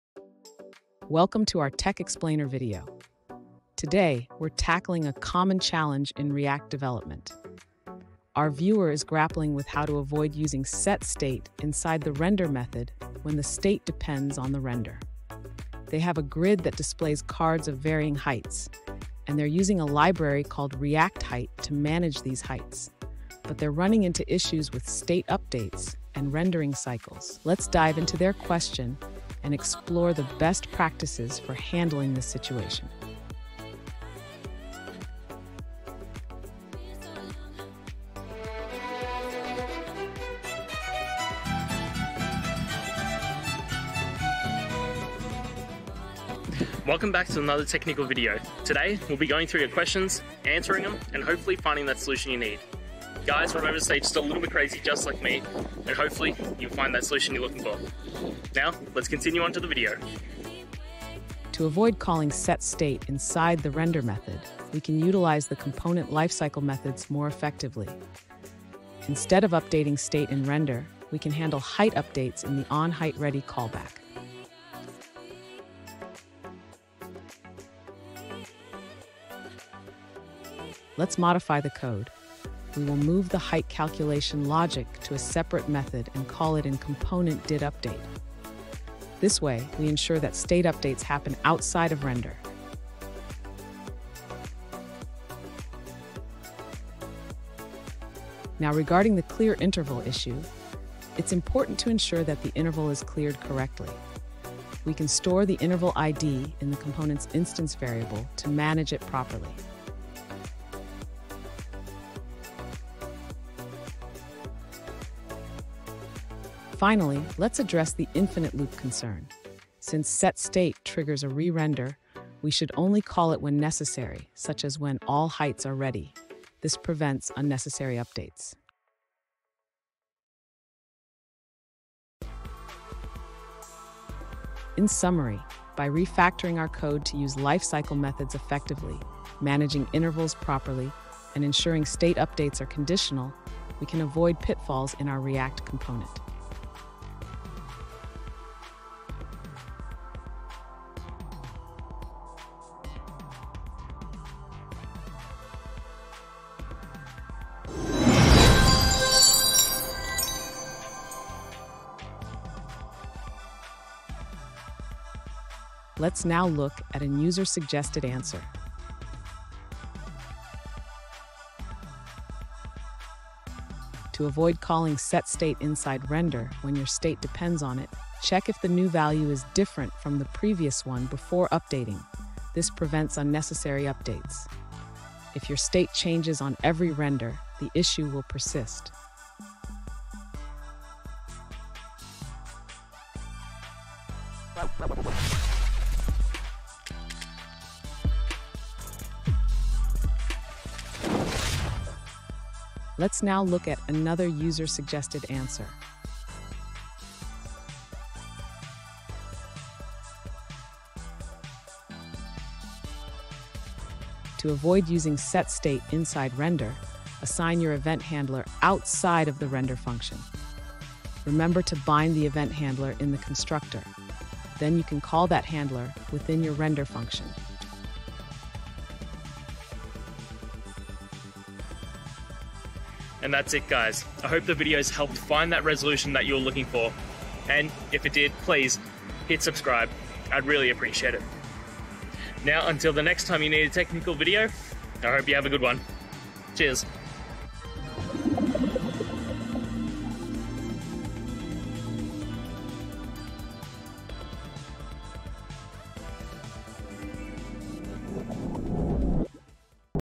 Unlock React Mastery: Avoid setState() in Render or Risk Your App's Performance!
In this video, we dive into a crucial aspect of React development that can significantly impact your application's performance: the use of `setState()` during the render phase. Many developers unknowingly introduce inefficiencies by updating state while rendering, leading to unnecessary re-renders and sluggish user experiences. Join us as we explore best practices to avoid this pitfall and unlock the full potential of React, ensuring your applications run smoothly and efficiently.

Today's Topic: Unlock React Mastery: Avoid setState() in Render or Risk Your App's Performance!

Related to